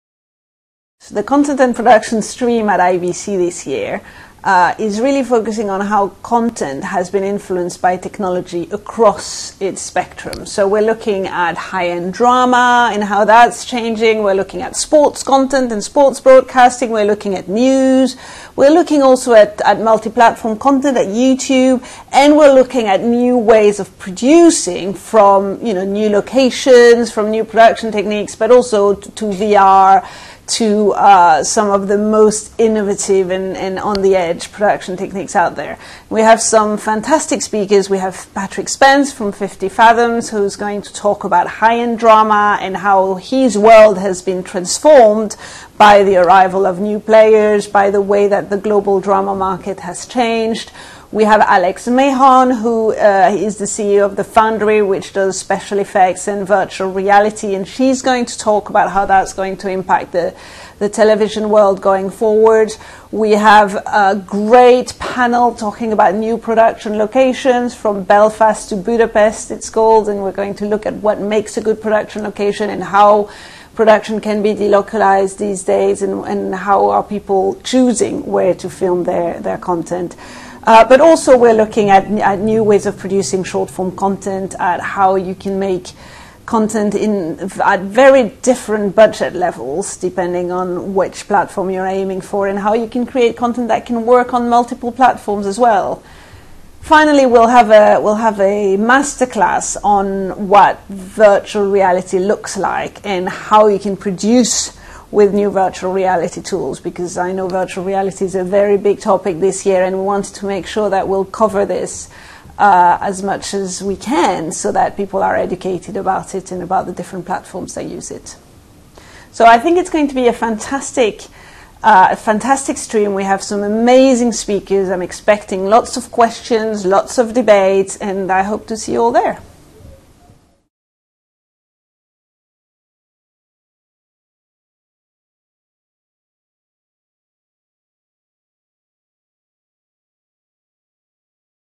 IBC2016: Content & Production Stream Claire Tavernier
The highlights of the Content and Production stream at the IBC 2016 conference. The stream will look at the impact of technology of content development and production across all genres.